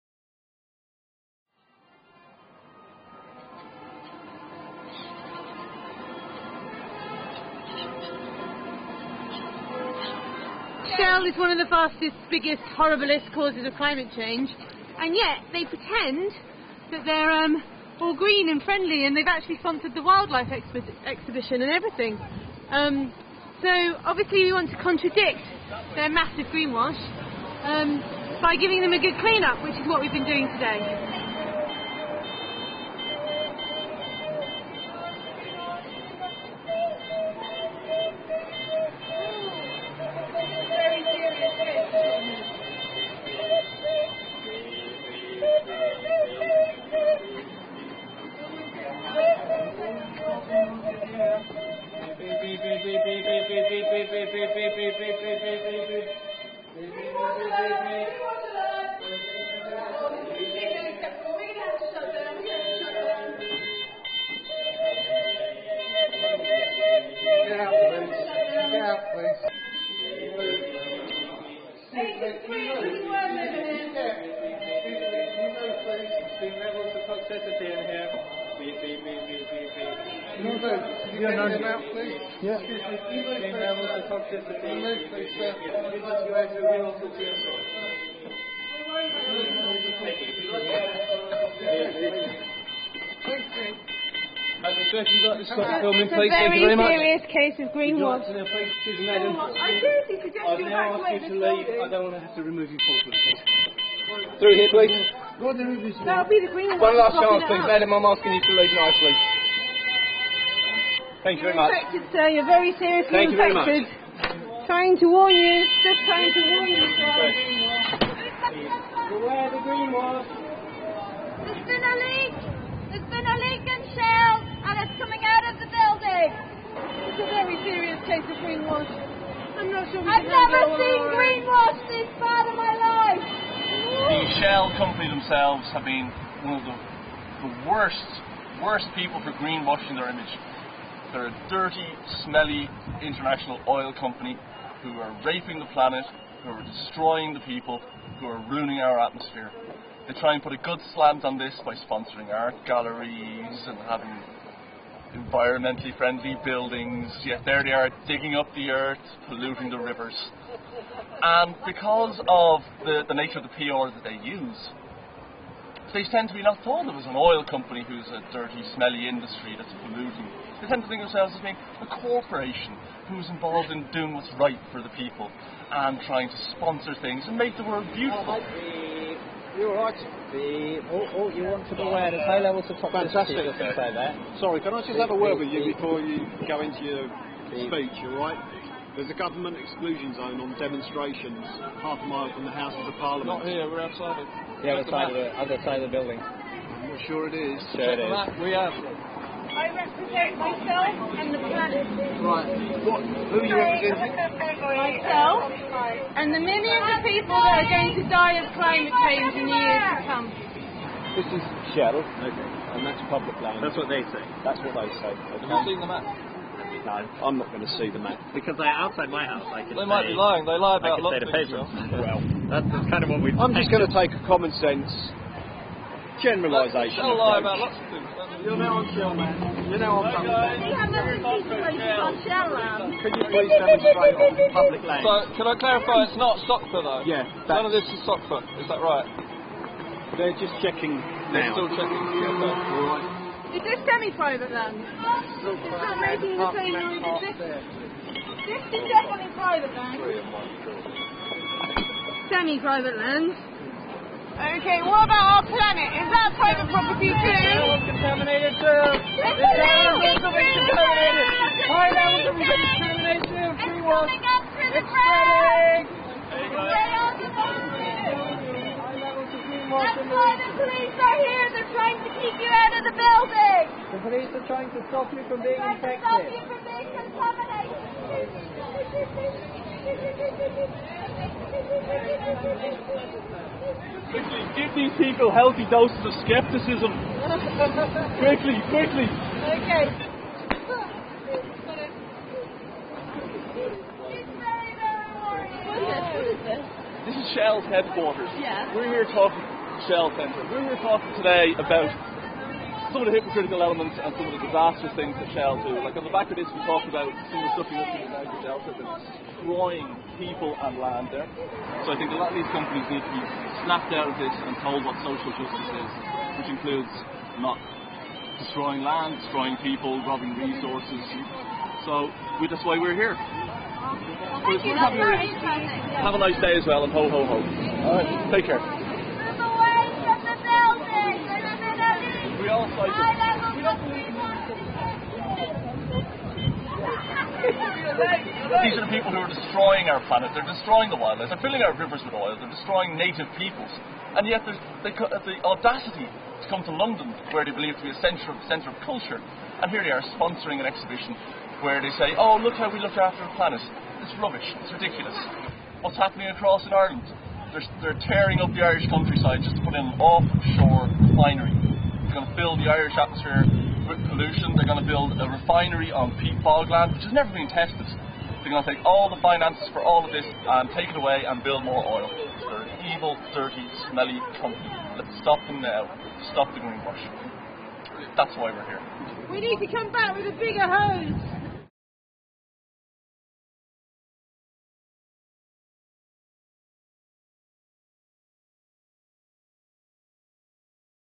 Rising Tide greenwash guerillas enter Shell HQ London
A band of greenwash guerillas entered the lobby of the london shell hq to highlight the massive amount of toxic greenwash emanating from the building. once ejected they continued their work outside, warning the public of the dangers of greenwash